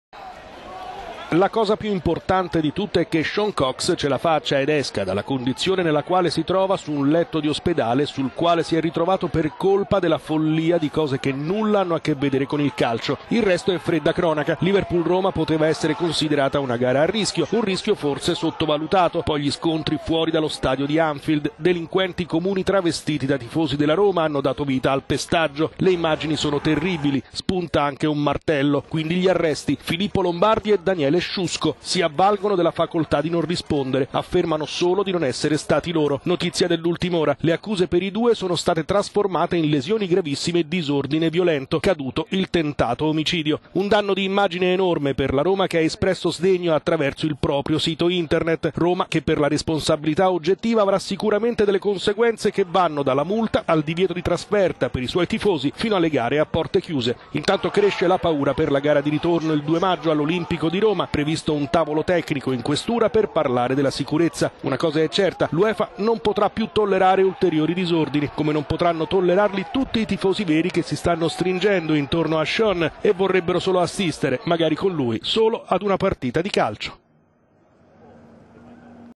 Roma-Liverpool: vertice sulla sicurezza, allerta nella capitale
La cosa piu' importante di tutte è che Sean Cox ce la faccia ed esca dalla condizione nella quale si trova su un letto di ospedale e sul quale si è ritrovato per colpa dalla follia di cose che nulla hanno a che vedere con il calcio. Il resto è fredda cronaca. …